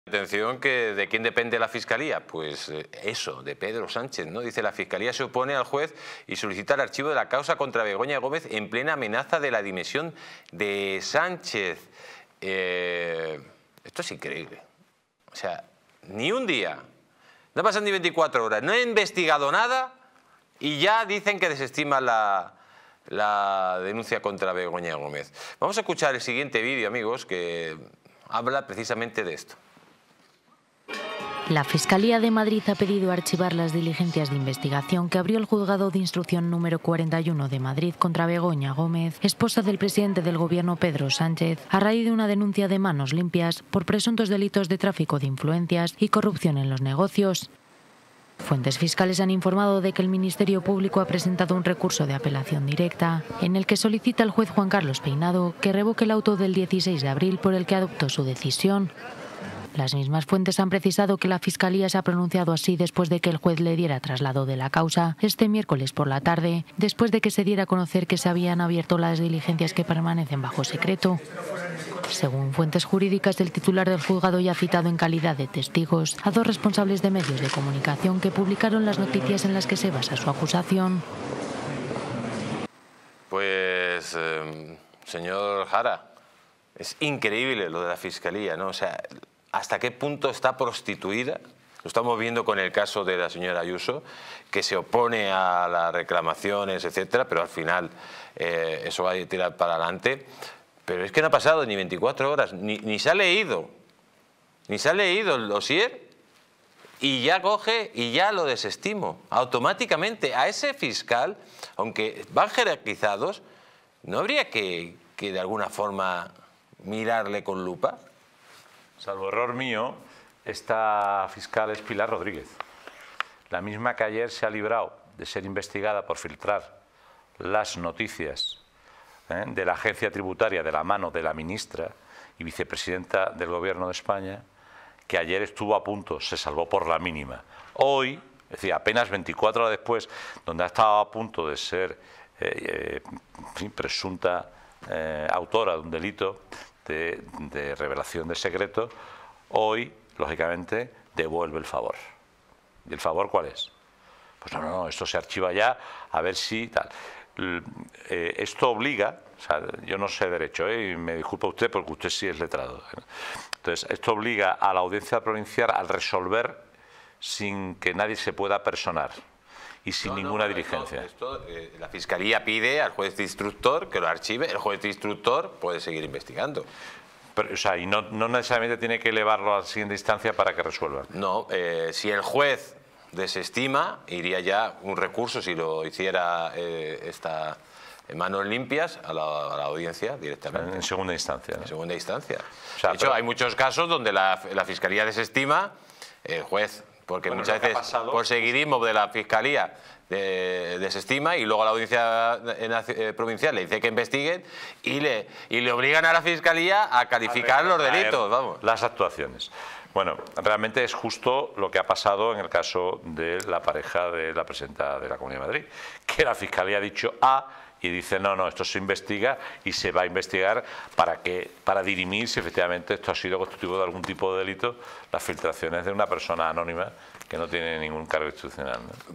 🔴Fiscalía pide archivar la causa contra Begoña Gómez🔴
¡Última hora! La fiscalía solicita archivar la causa contra Begoña Gómez en este video exclusivo de Distrito TV. ¡No te lo pierdas! 🔴
🔴Conviértete en miembro de este canal para disfrutar de ventajas: